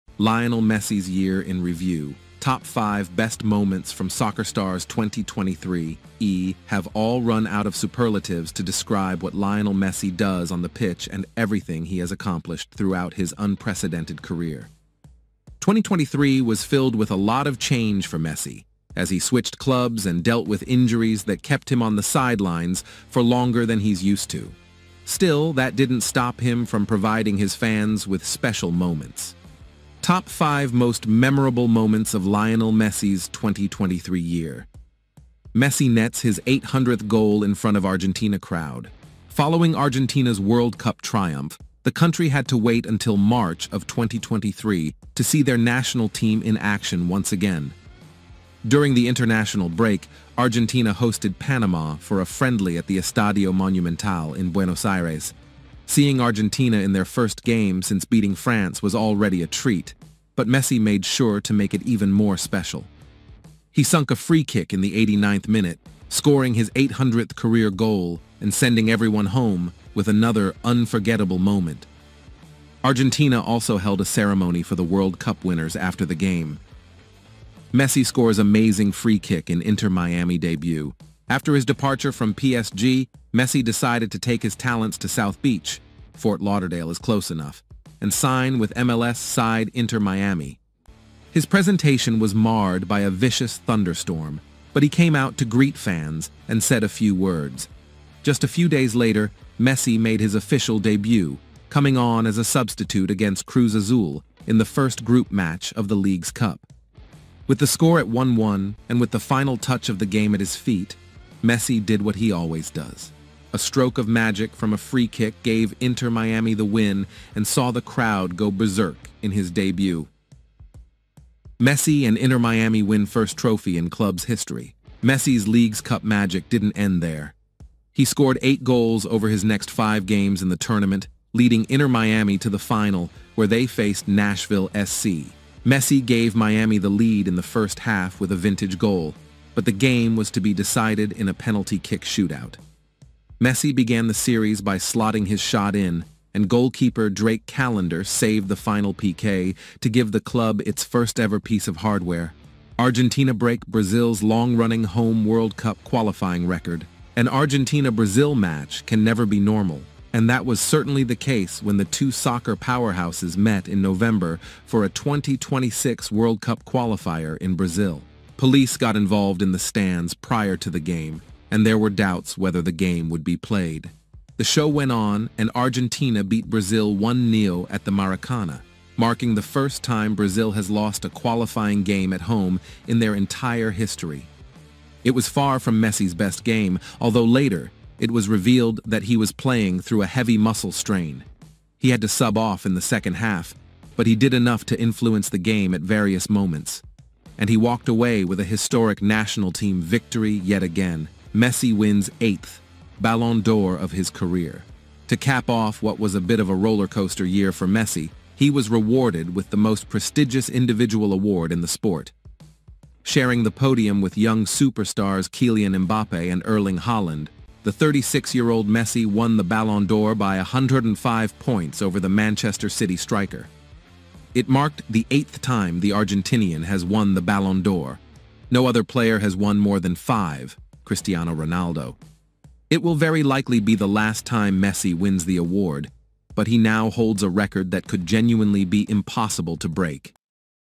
Lionel Messi's Year in Review! Top 5 Best Moments From Soccer Star's 2023 |Messi's Best Achievements
Lionel Messi's Year in Review ! Top 5 Best Moments From Soccer Star's 2023 | Messi's Best Moment 23